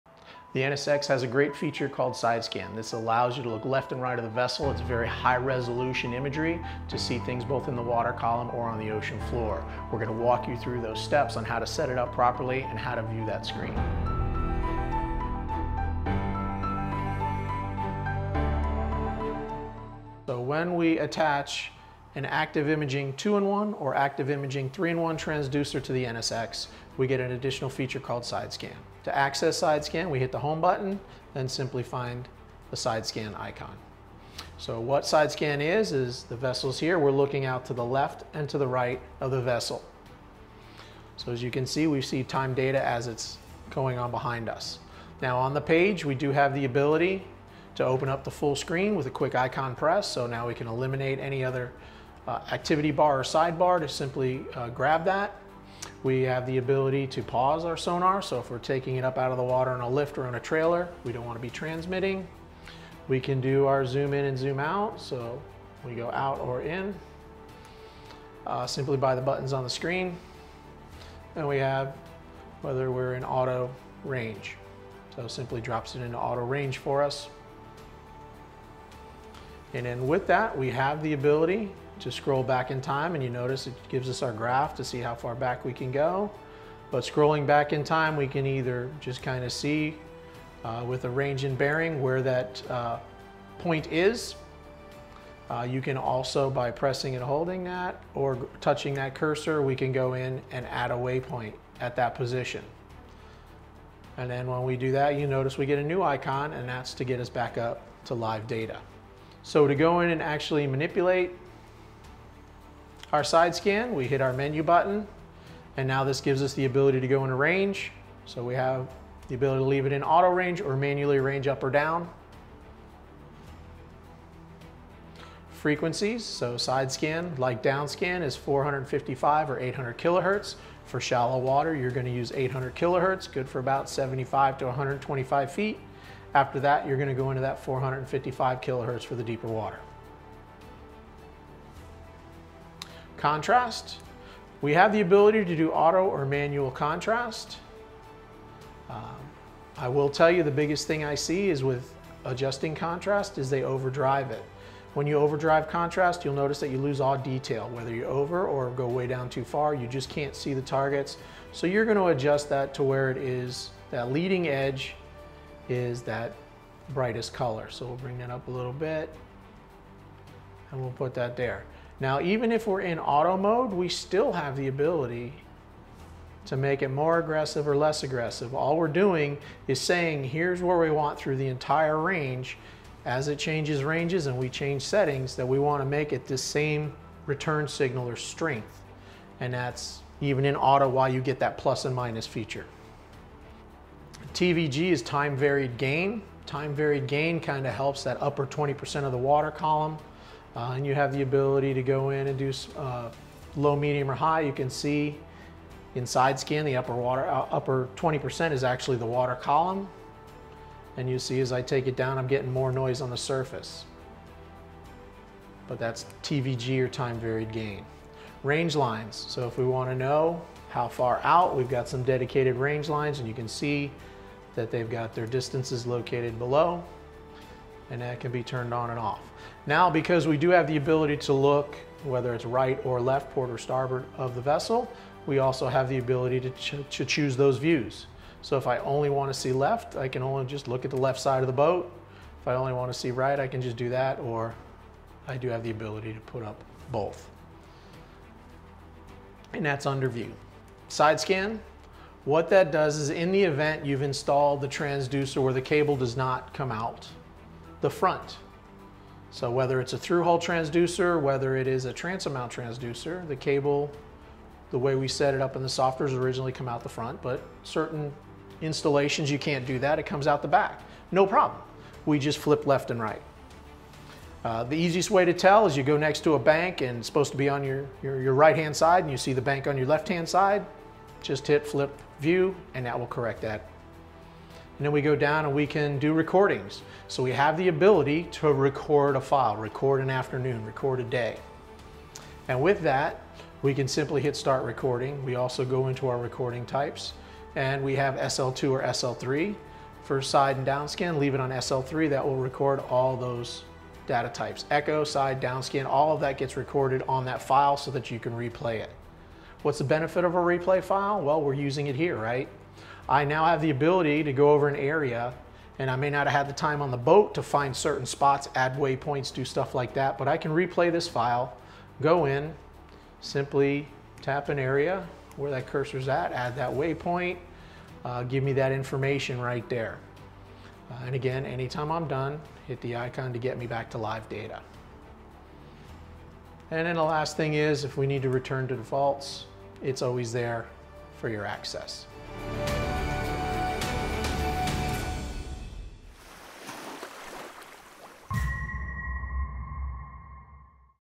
Simrad | NSX SideScan Guide
A walkthrough of the SideScan feature on the NSX™. SideScan provides high-resolution views left and right of the boat, both in the water column and the ocean floor.  Sean explains how to set it up properly and how to view the screen.

00:00 Intro NSX SideScan Guide
00:19 Accessing SideScan
00:34 How to use SideScan
01:54 Adjusting SideScan settings
01:58 Adjusting range
02:04 Changing frequencies
02:22 Adjusting contrast
03:15 Adjusting TVG
03:39 Setting range lines
03:50 Changing view
04:12 Flipping the view
04:44 How to record SideScan
05:38 Restoring default settings
05:44 Outro